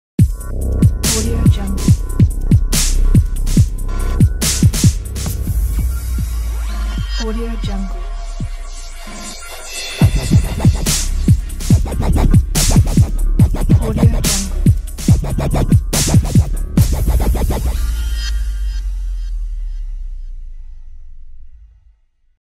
Prime Glitch Intro | Apple Motion Templates
Compatible with Apple Motion 5.3 and higher versions.
Compatible with Latest FCPX version
Project Have 5 Video/Image Dropzones and 6 Text Containers
Video help included for customizing the project.
Images/Videos used are for Preview Purpose Only. Not Included in the Project.